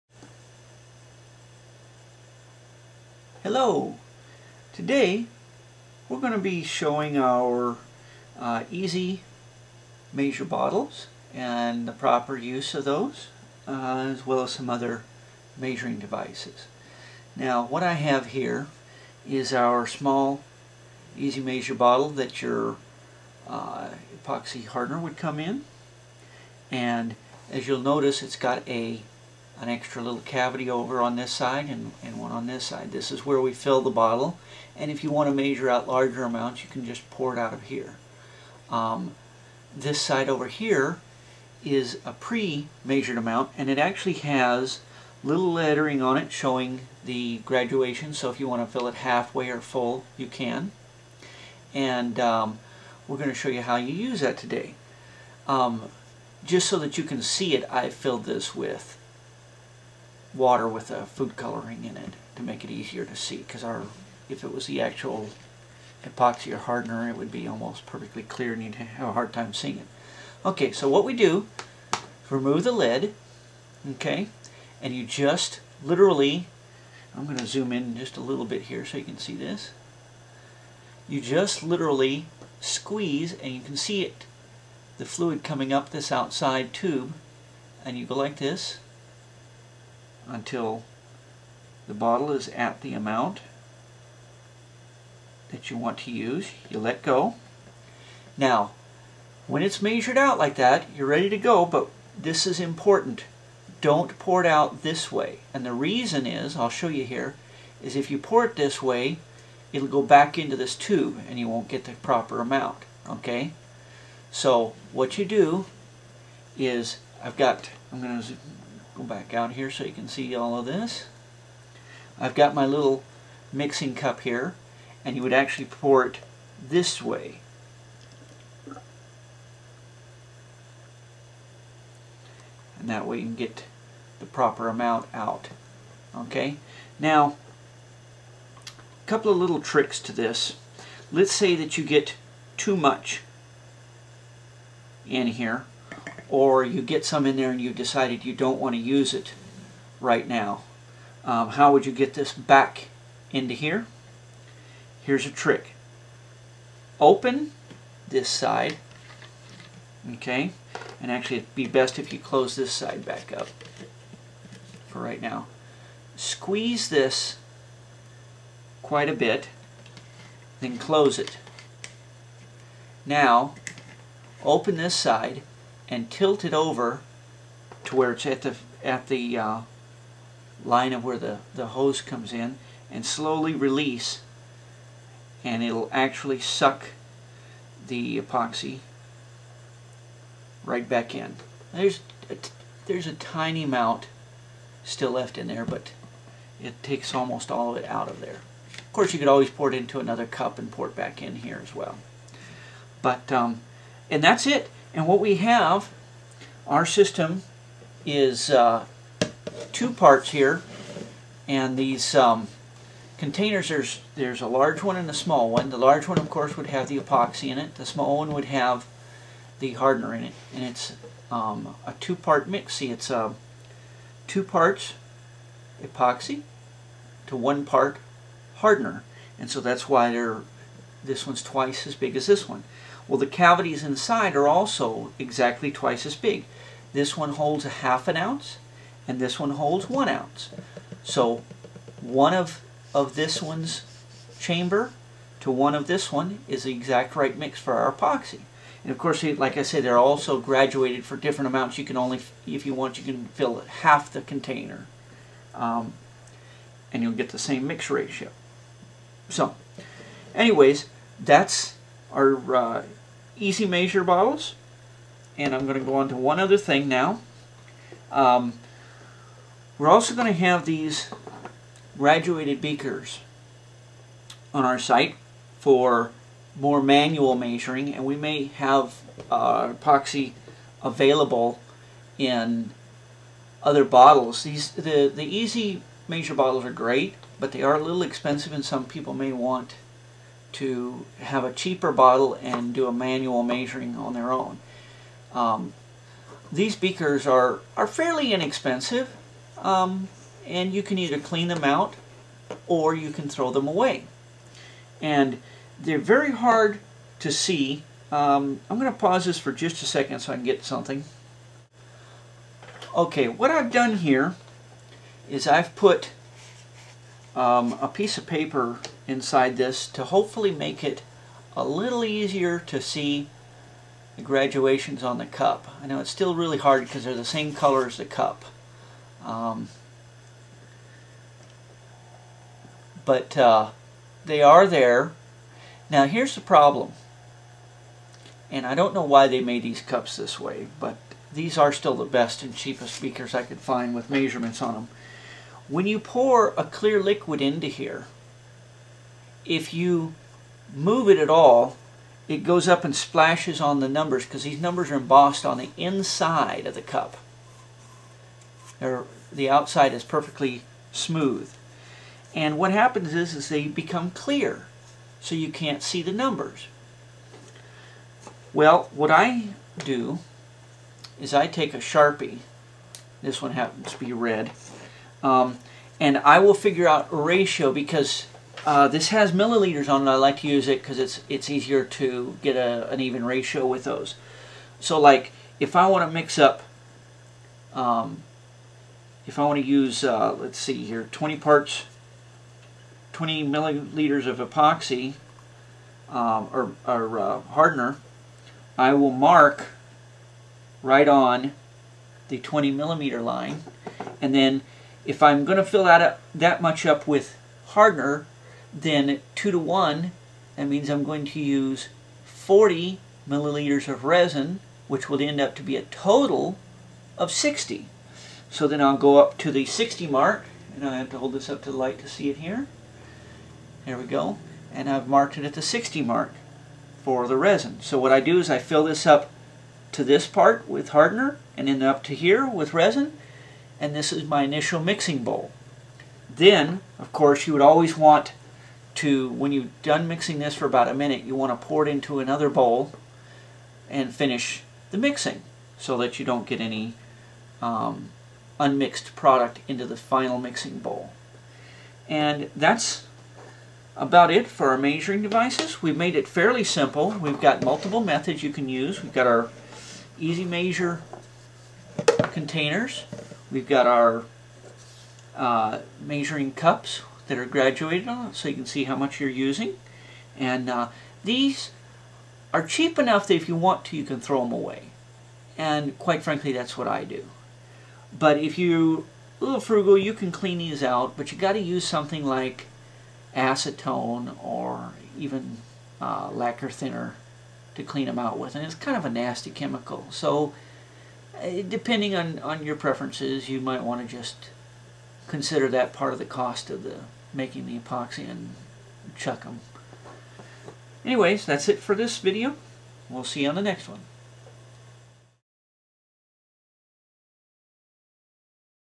Using EZ-Measure bottles and graduated beakers
This is a tutorial showing the proper use of our EZ-Measure bottles and graduated beakers for proper proportions of epoxy and hardener.